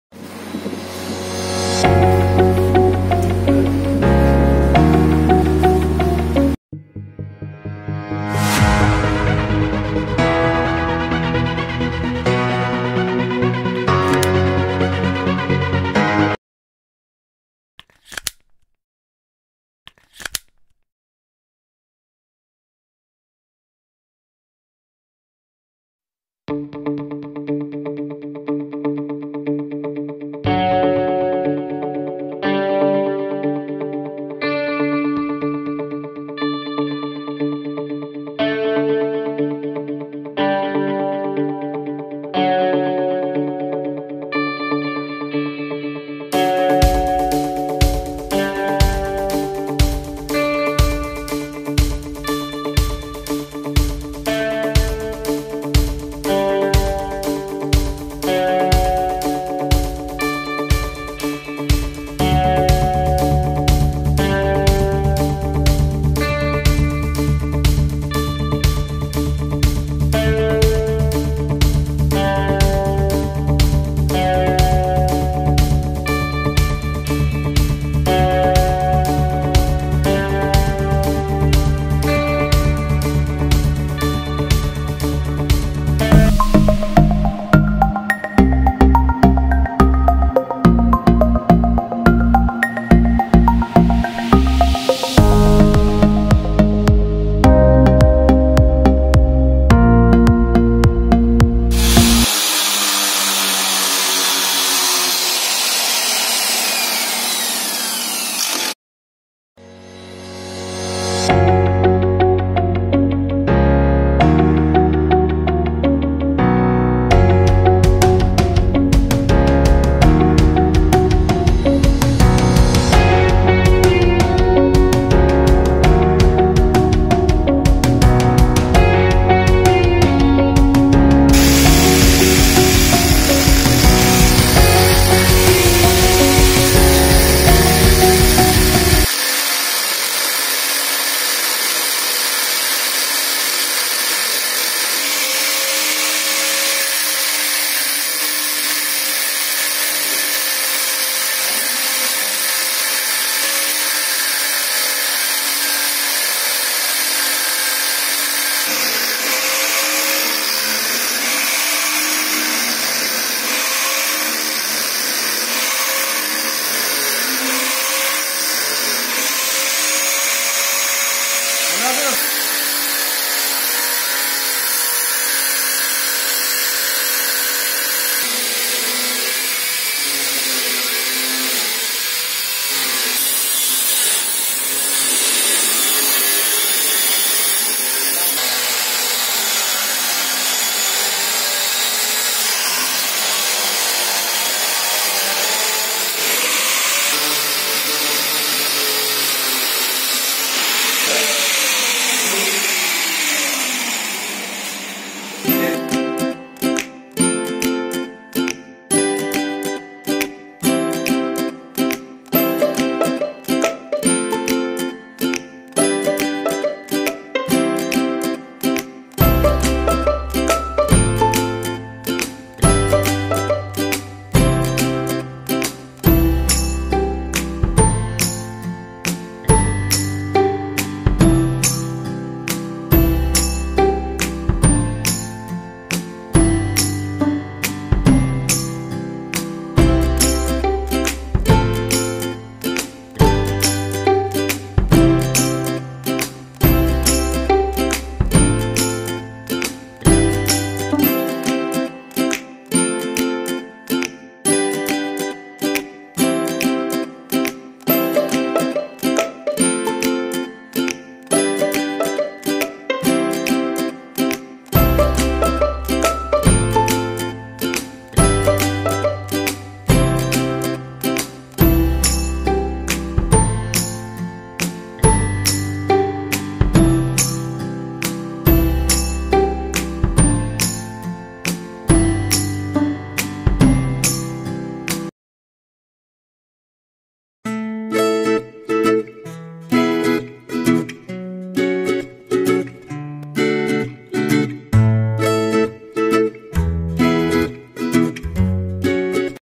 Distance Protection Relay Installation| Micom P442
This video shows how to install Numerical Distance Protection Relay at Control Panel of Transmission Line.